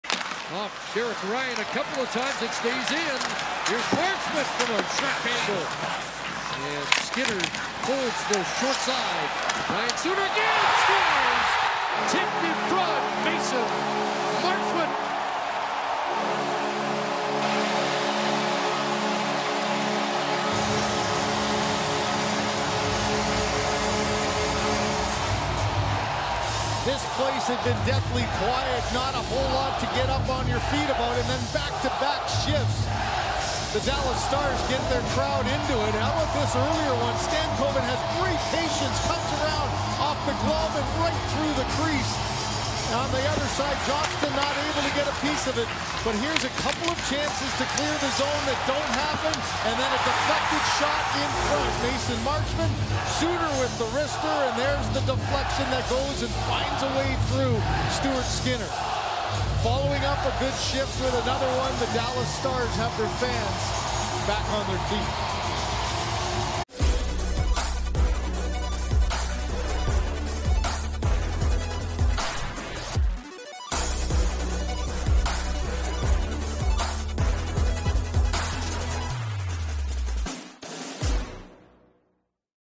Mason Marchment Redirects Ryan Suter Point Shot For Cheeky Goal
Watch as Dallas Stars forward Mason Marchment scores a goal during the third period of Game 2 by redirecting a point shot from Ryan Suter past Edmonton Oilers goaltender Stuart Skinner.